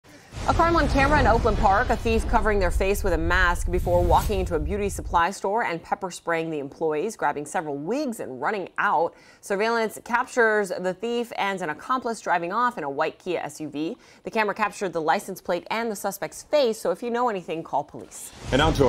Thief caught on camera stealing supplies from Oakland Park beauty shop
Police are searching for a suspect that was caught on camera stealing beauty supplies from a store in Oakland Park.